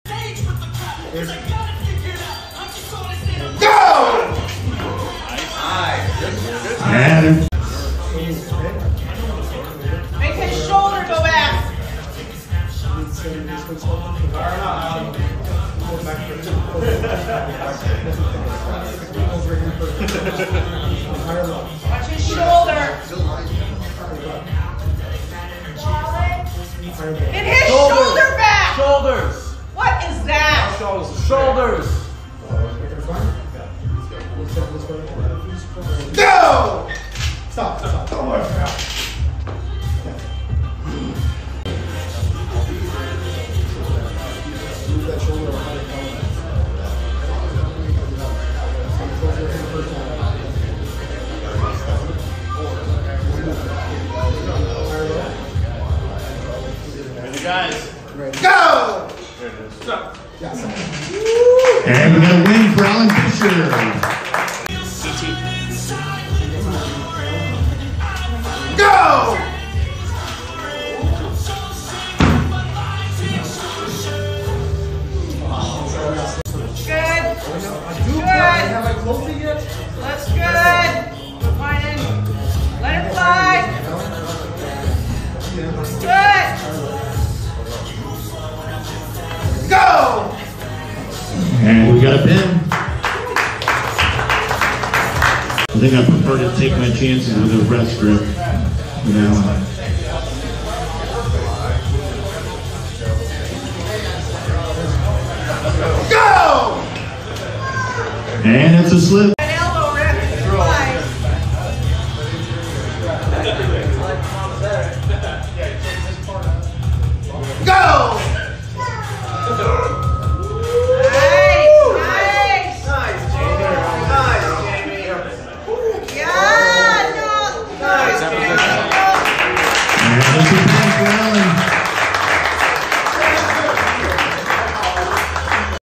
26 × World Champion Allen Fisher VS 16 Year Old Jamie Barrett | Best of 5, New Gen VS Old Gen.
26 × World Champion Allen Fisher VS 16 Year Old Jamie Barrett | Best of 5, New Gen VS Old Gen.